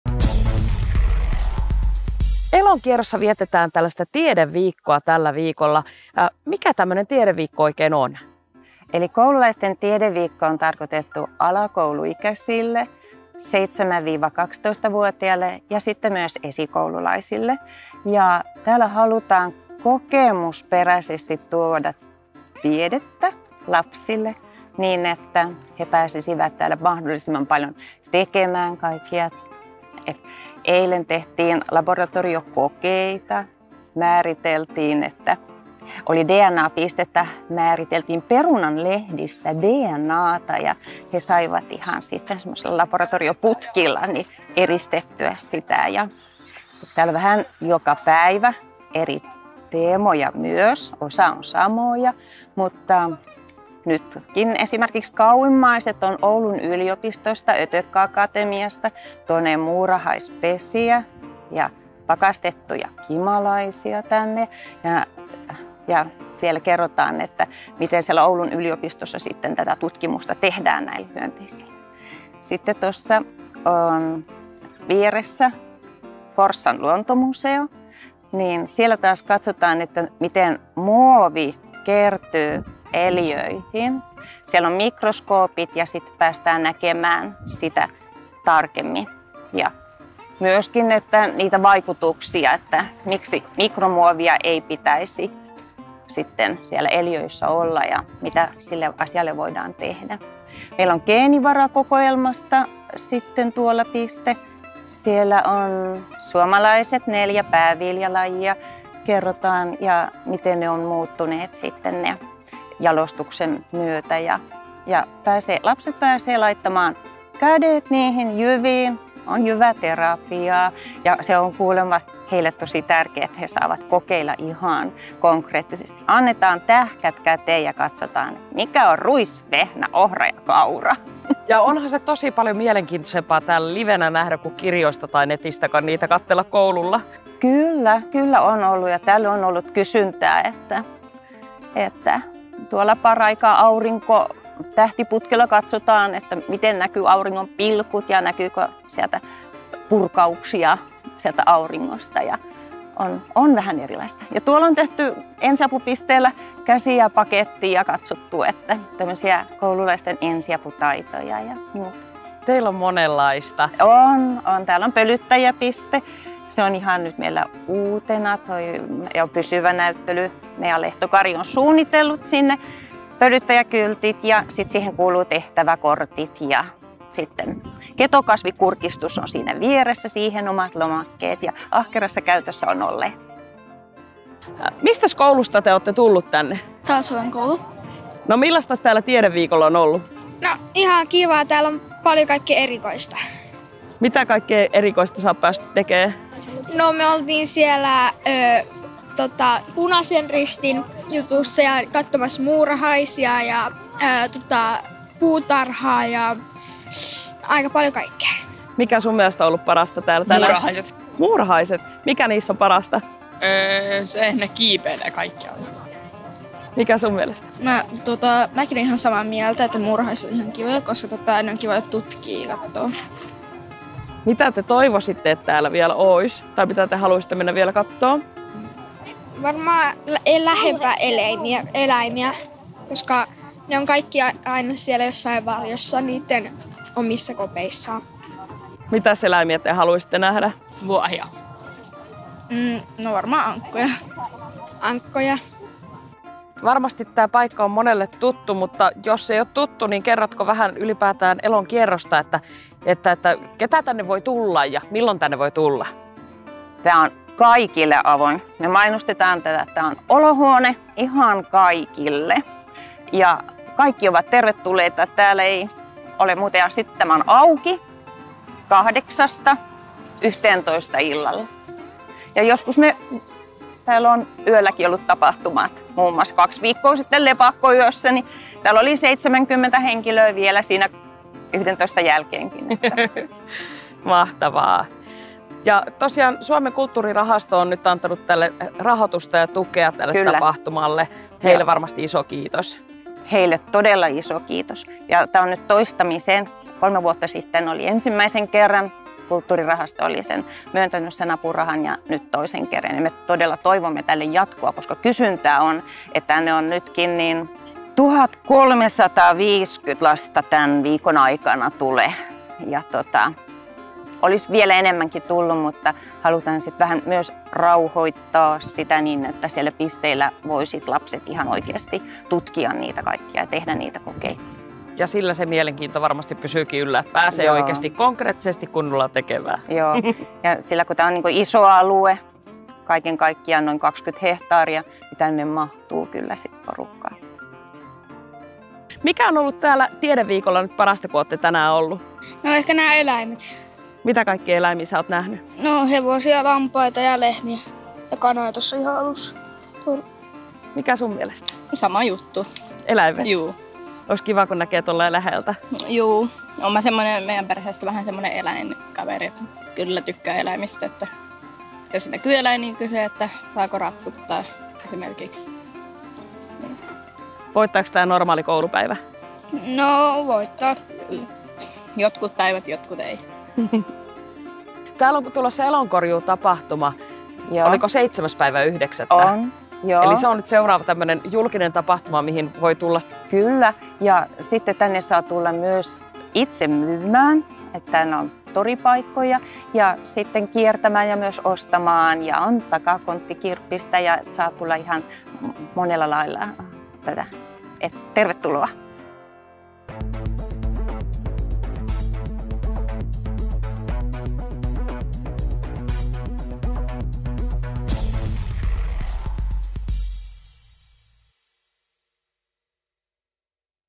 Hätäset - Tiedeviikko Elonkierrossa
Jokioisten Elonkierrossa vietettiin viime viikolla Tiedeviikkoa. Hätäset poikkesi paikalla jututtamassa niin tekijöitä kuin kävijöitäkin.